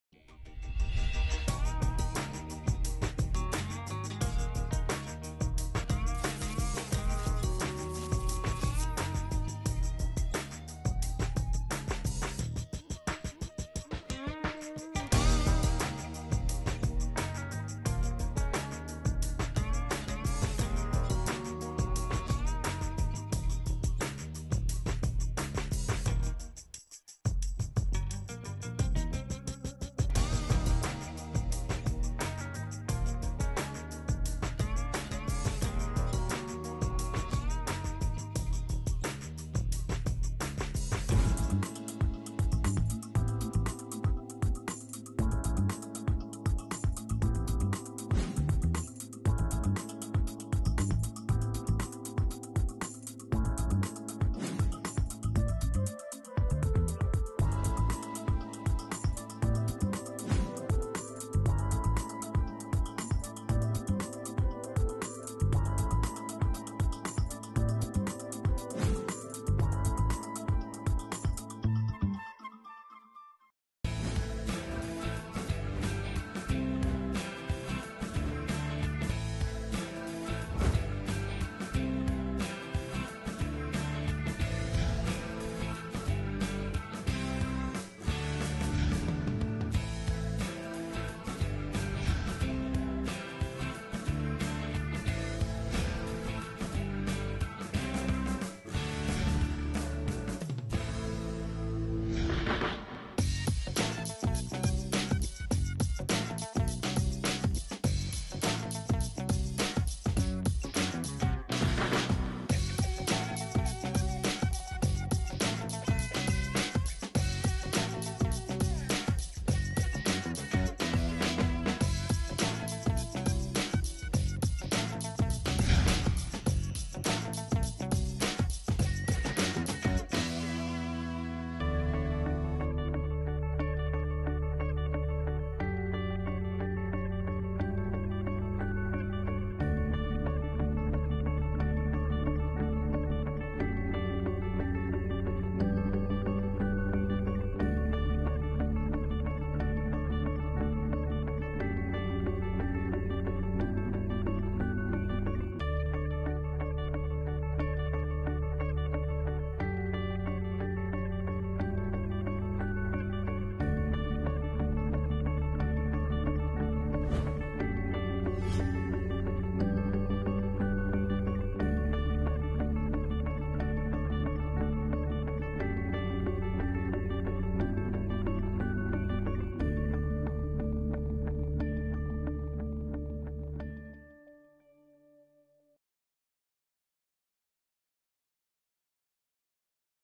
The New Food Innovation Centre
AZTI-Tecnalia presents its new headquarter in the Bizkaia Technology Park. New facilities, new technologies and new services...